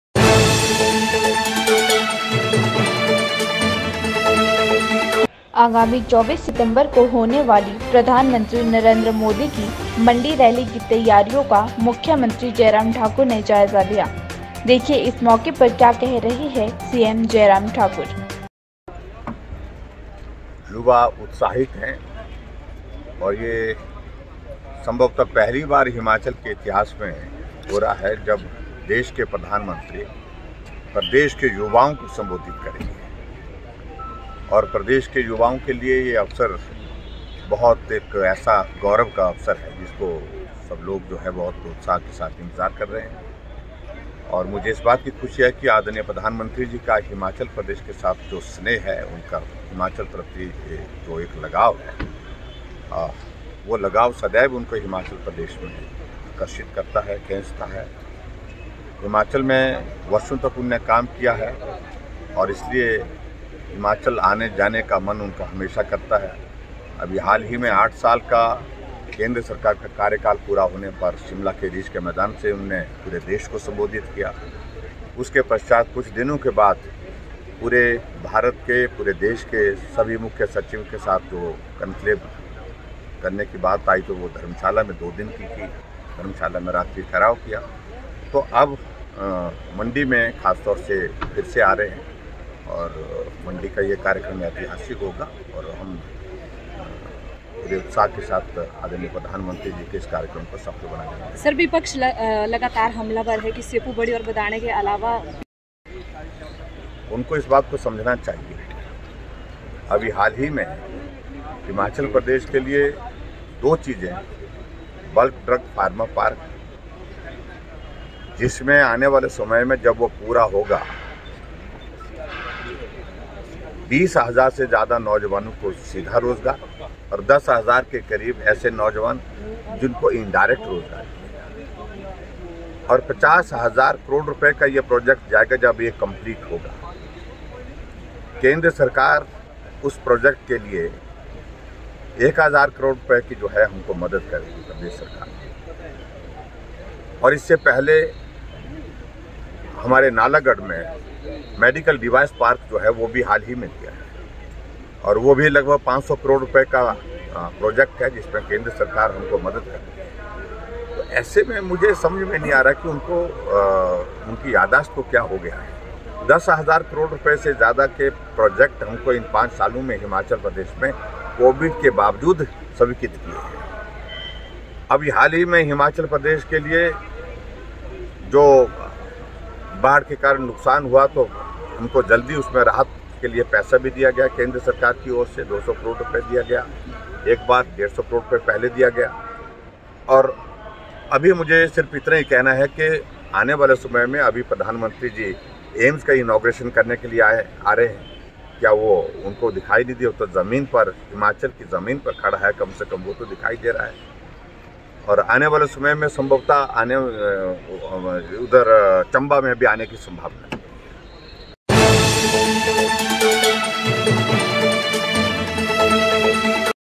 मुख्यमंत्री ने पडडल मैदान में होने बाली पीएम रैली का लिया जायजा
आगामी 24 सितंबर को होने बाली प्रधानमंत्री नरेंद्र मोदी की मंडी रैली की त्यारियों का मुख्यमंत्री जयराम ठाकुर ने जायजा लिया।देखिए इस मौके पर क्या कह रहे हैं सीएम जयराम ठाकुर